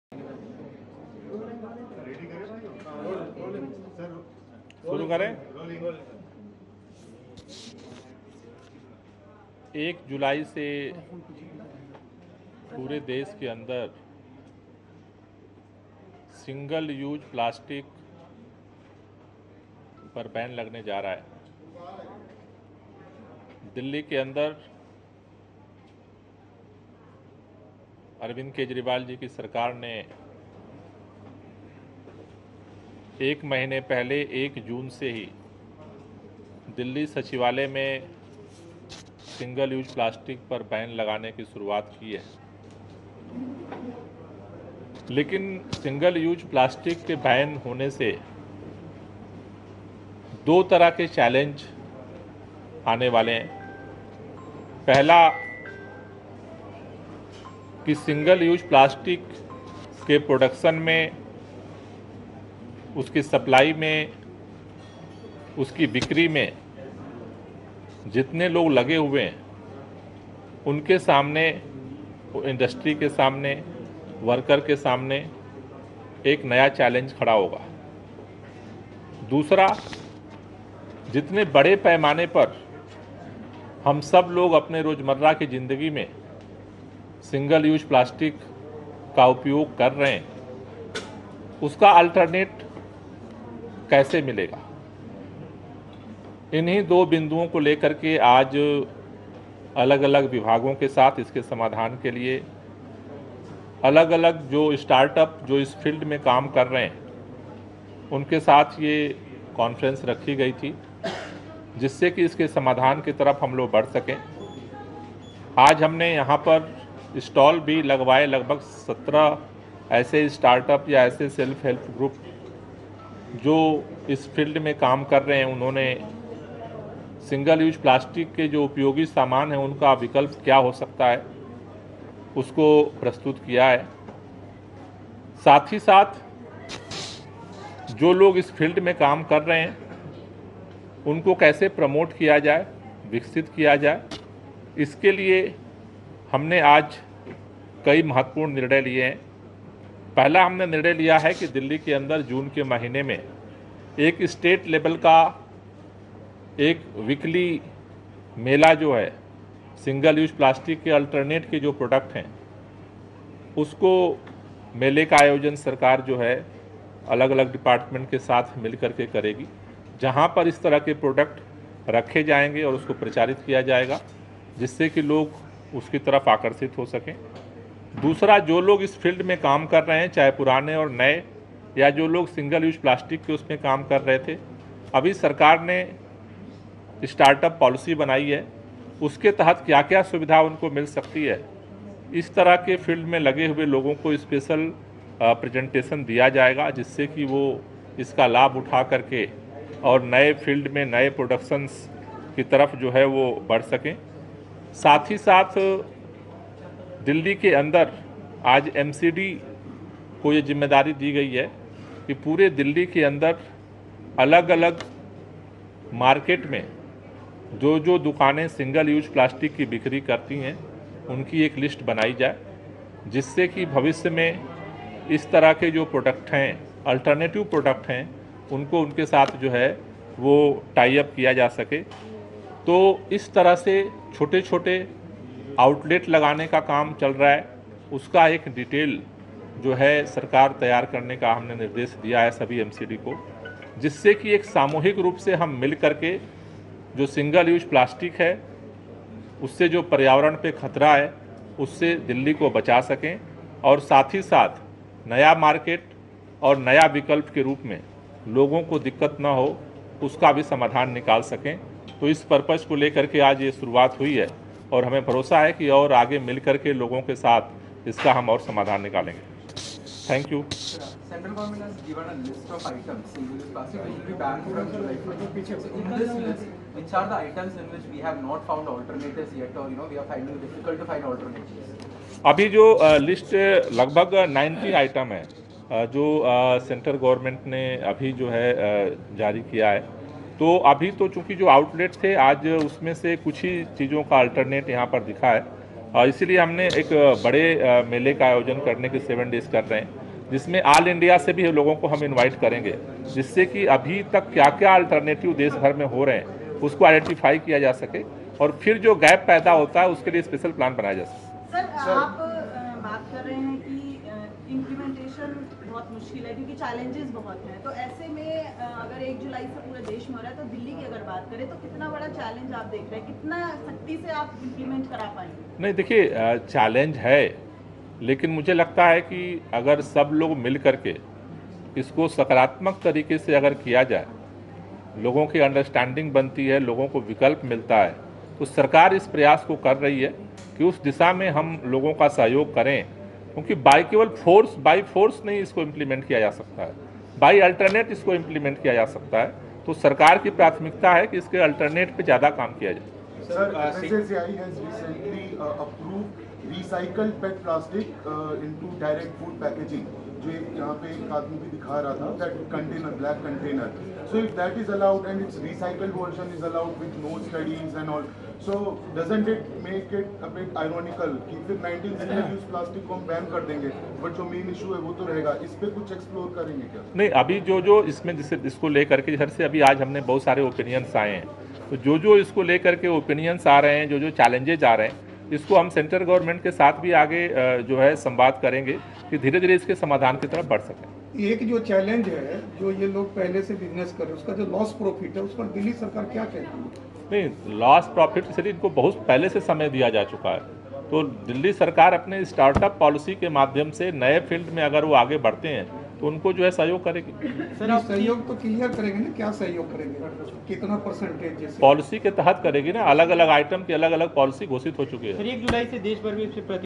Stv National || प्लास्टिक का विकल्प बनाने वाली कंपनियों की ऐसे मदद करेगी Kejriwal Govt Gopal Rai AAP
stvnational प्लास्टिक का विकल्प बनाने वाली कंपनियों की ऐसे मदद करेगी Kejriwal Govt Gopal Rai AAP Delhi